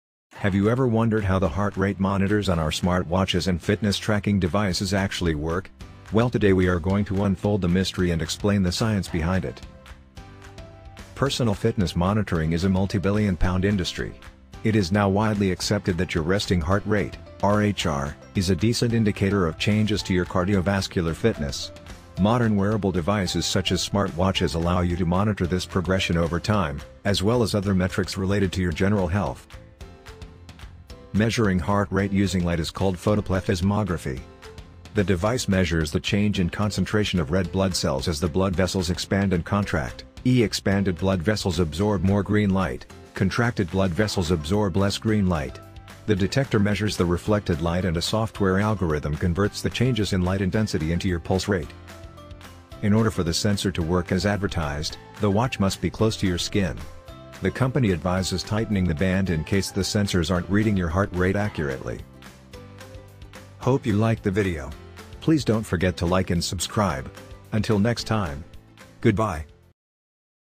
How Smart Watches Measure Heart Rate
Ever Wondered how your Apple Watch, Fitbit or any other fitness tracking devices measure your heart rate just by wearing on your wrist. This video try to explain just that in simple terms.

how smart watches work
Do smart watches accurately measure heart rate?
Do smart watches actually work?